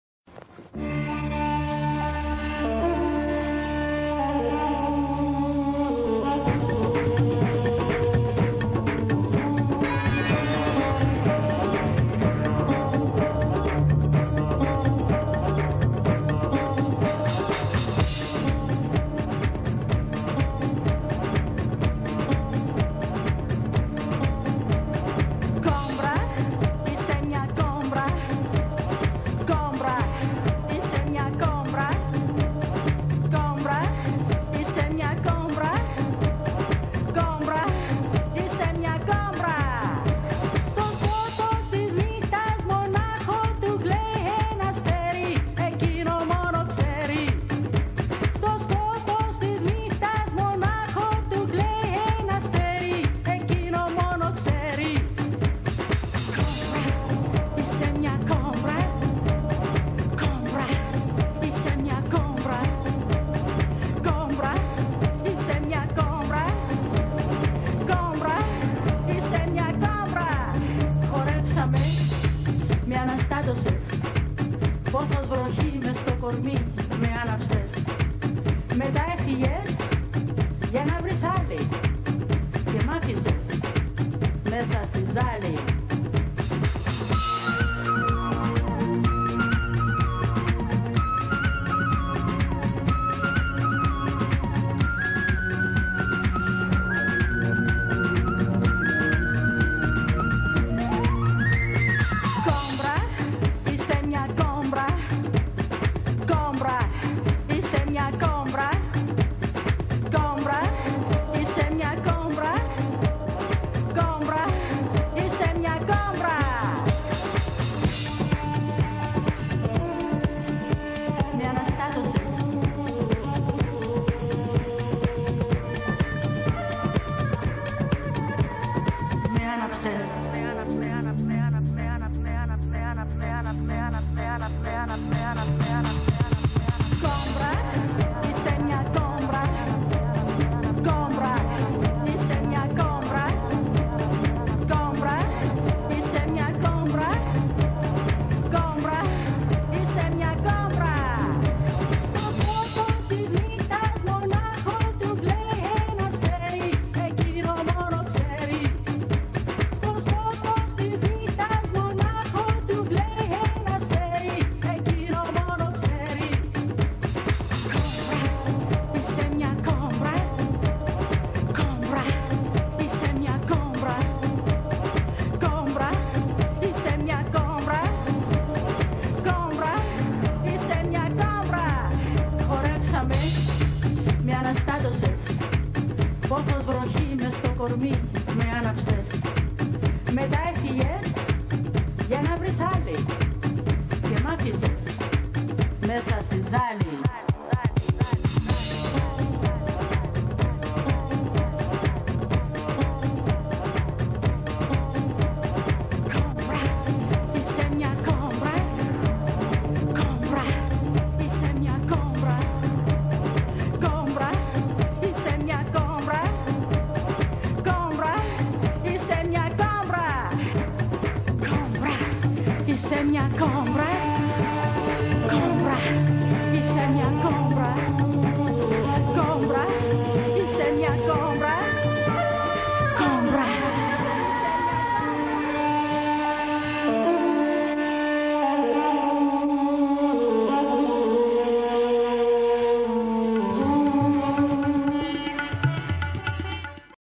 HHI 2013 COBRA Edition | Funny Video
Hip-Hop International Greece 2013 HD
Cobra eisai mia cobra - Κομπρα εισαι μια κομπρα

ΤΟ ΒΙΝΤΕΟ ΕΙΝΑΙ ΧΙΟΥΜΟΡΙΣΤΙΚΟΥ ΠΕΡΙΕΧΟΜΕΝΟΥ.
ΤΑ ΚΑΝΟΝΙΚΑ ΒΙΝΤΕΟ ΤΩΝ ΟΜΑΔΩΝ ΜΠΟΡΕΙΤΕ ΝΑ ΤΑ ΔΕΙΤΕ ΕΔΩ.